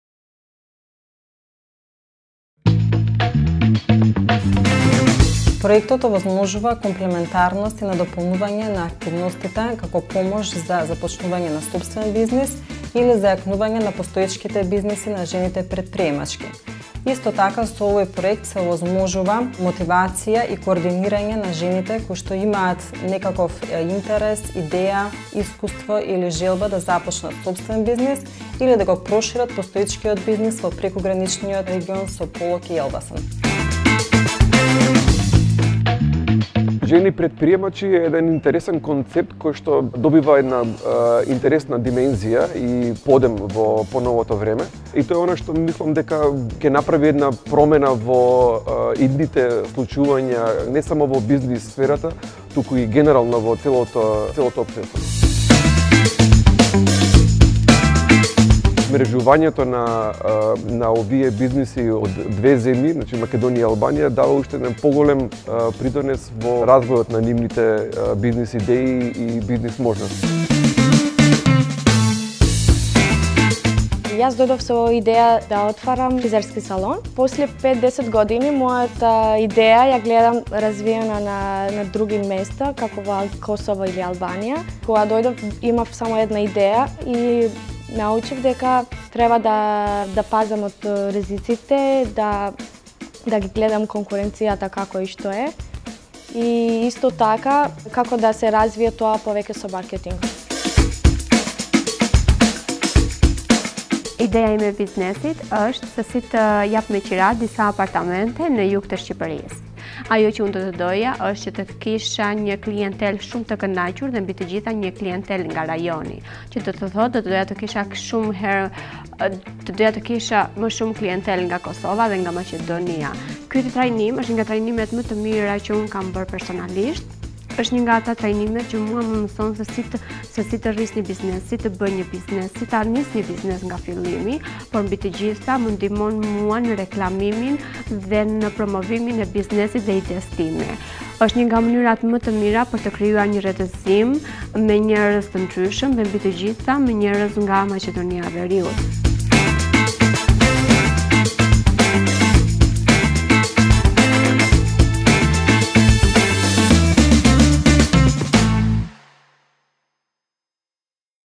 Обуки за развој на бизнис планови на жени претприемачки
“Microenterprise development project for women in the cross-border area” is an EU funded project from the IPA II Cross border cooperation programme between North Macedonia and Albania.